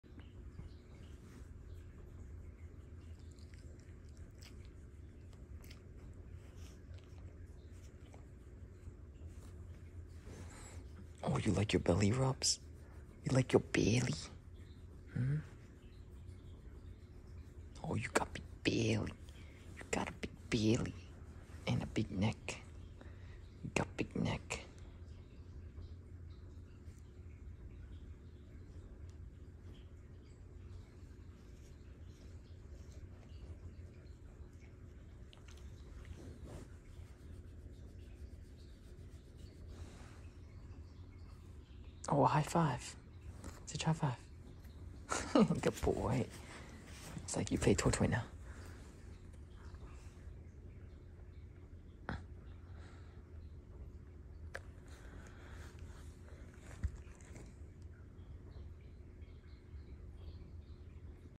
Corgi Getting Love On The Couch
Corgi Stitch getting love from Dad while being lazy on the couch